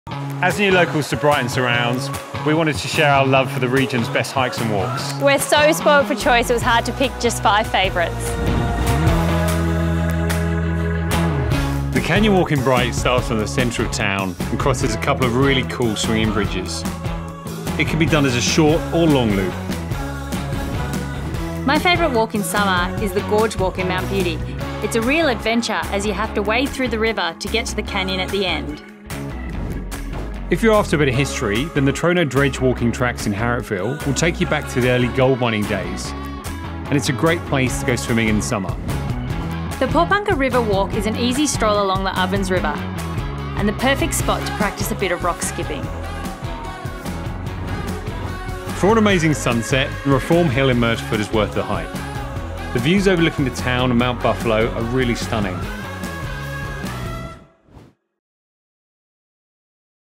Bright & Surrounds - Favourite Walks
Bright & Surrounds offers so many beautiful walks.  No matter the distance you feel like walking or the scenery you wish to surround yourself with; we have something for you.
Meet locals Lucy and Tom, they want to share a few of their favourite tracks around our region….

Fun Fact; Lucy has been running the trails around our region for the past 12 months, training physically and mentally to take on her dream of running the length of New Zealand, top to bottom!  3000km in 70 days.   She is inspirational to anyone wanting to follow a dream.  You can follow her progress on Instagram @adventurous_lucy